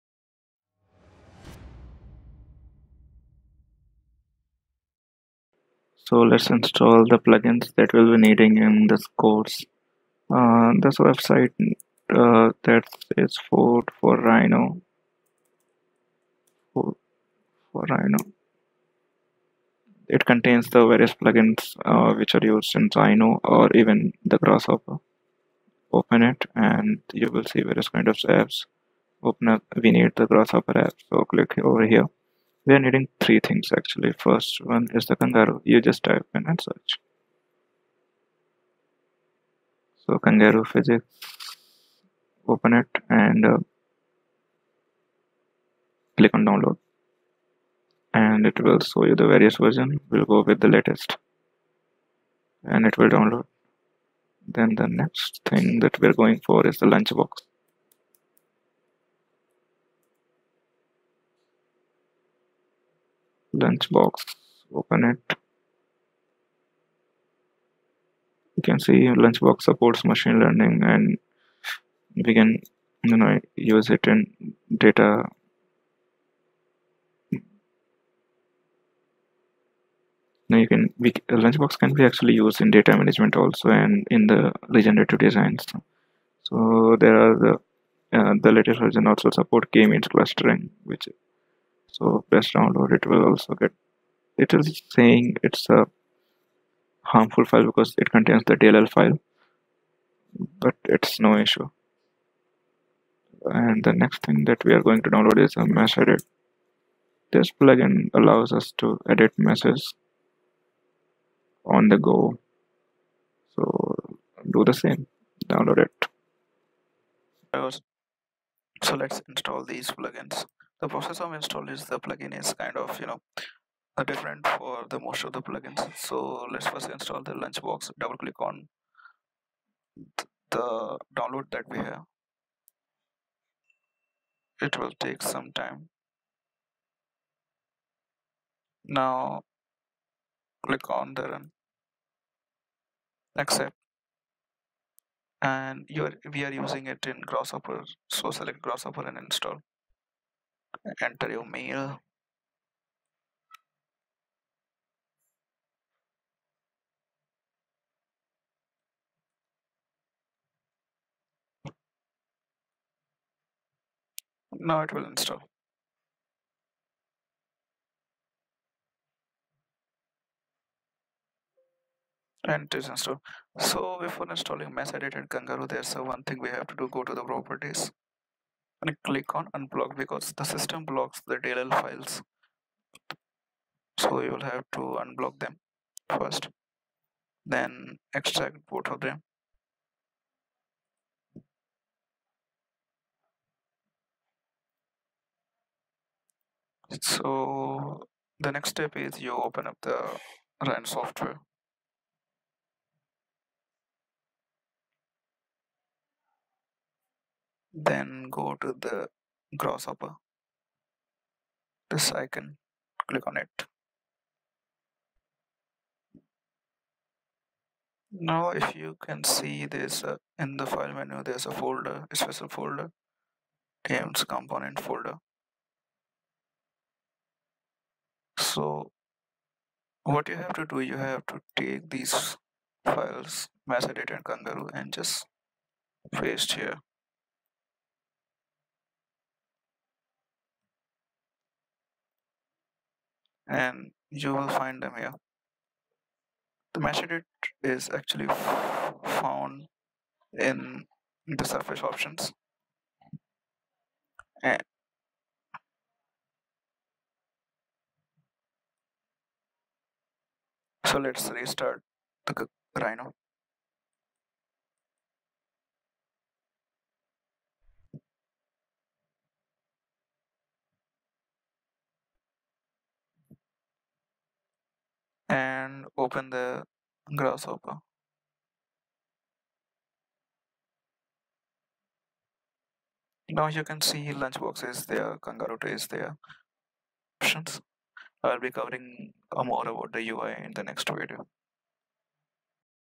Installing GrassHopper plugins ( kangaroo, lunchbox, meshedit) #3 GrassHopper tutorial series
For best sound quality, try earphones/headphones.
This video contains step by step process to install grasshopper plugins. Ask any doubts if you have in comments.